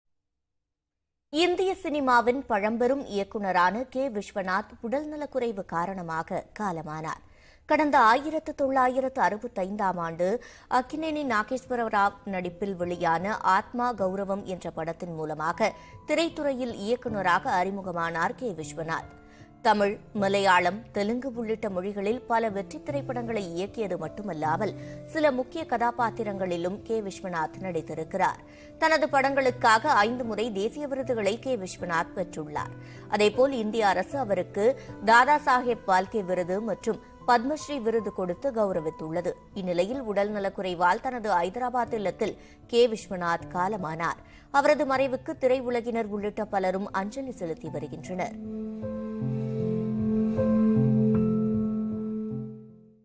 Indian Cinema | பழம்பெரும் இயக்குனரான K Vishwanath காலமானார் | Director | Tamil News
இயக்குநர் கே விஸ்வநாத் உடல் நலக்குறைவு காரணமாக காலமானார். செய்திகளை துல்லியமாகவும் துரிதமாகவும் வழங்கும் தமிழகத்தின் முதன்மை செய்தி தொலைக்காட்சி உங்கள் நியூஸ்18 தமிழ்நாடு